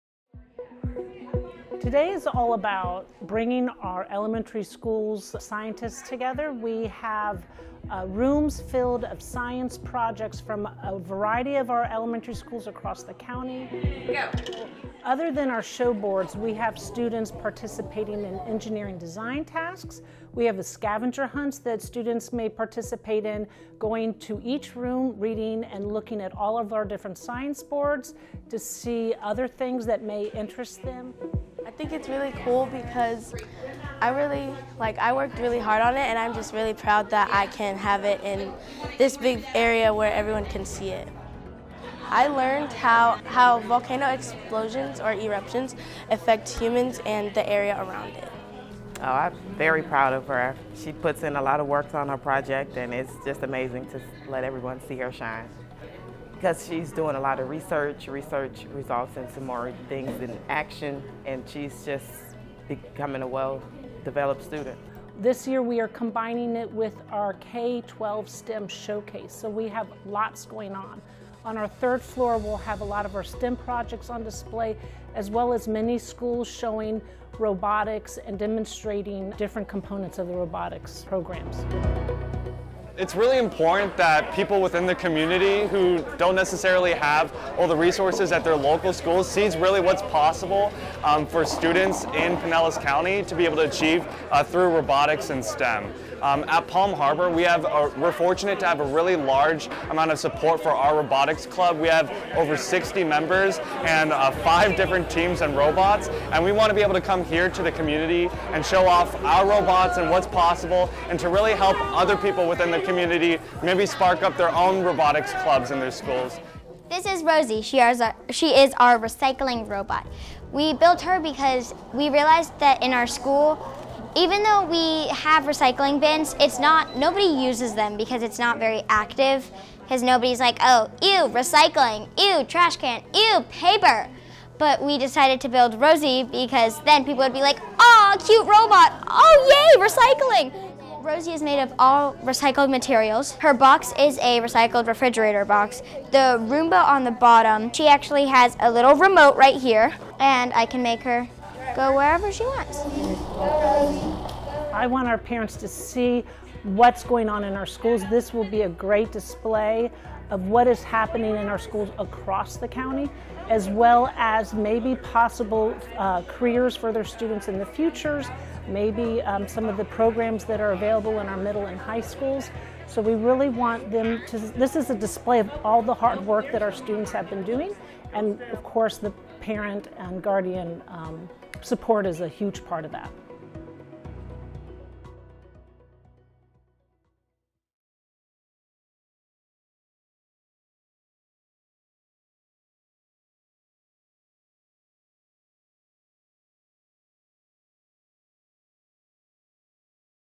Young Scientists show off their talents at PCS Elementary Science Showcase and STEM Expo
Let's visit the 2022 Elementary Science Showcase and STEM Expo! Students are involved with multiple interactive science centers with engineering design tasks and robotics while showing off their STEM and Science projects.